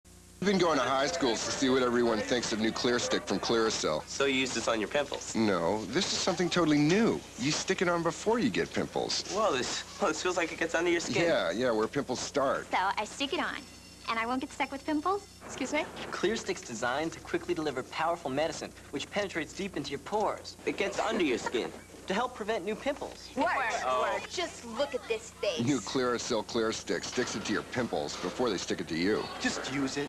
1991 Clearasil Clearstick "High School" TV Commercial
Colter Rule with the VO

From Colter:
"Colter Rule  Hahaha...yea...I did the voiceover for it...nice gig..nice spot"

This commercial aired in 1991 during an MTV 1/2 Comedy hour show. MTV, Music Television is a youth oriented music cable channel.